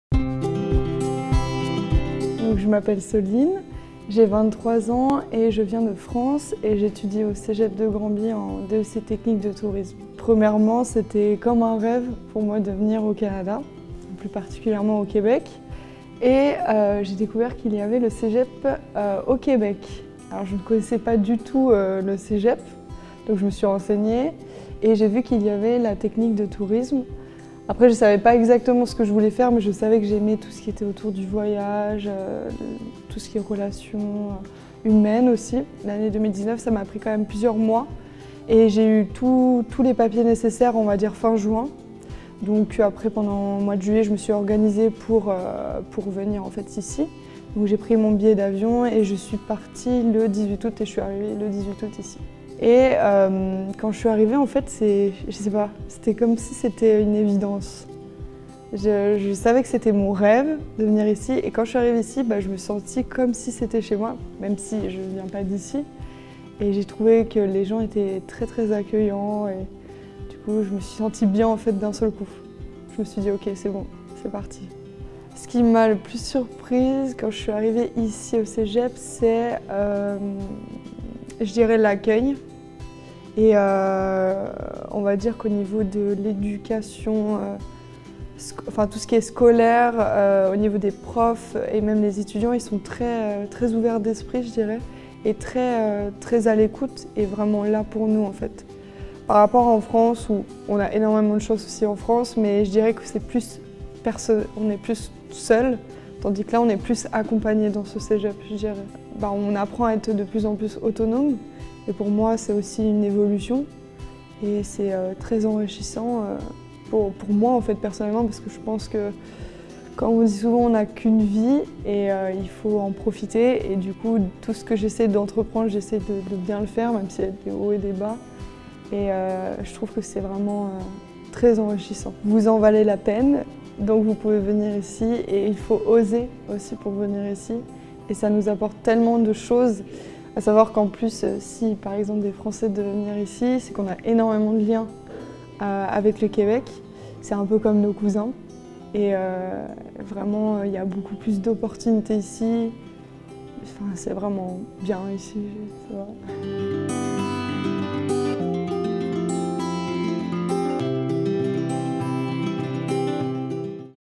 Soline - étudiante de l'international en Techniques de tourisme au Cégep de Granby
Soline Marty, étudiante de l'international, nous entretient de son expérience québécoise.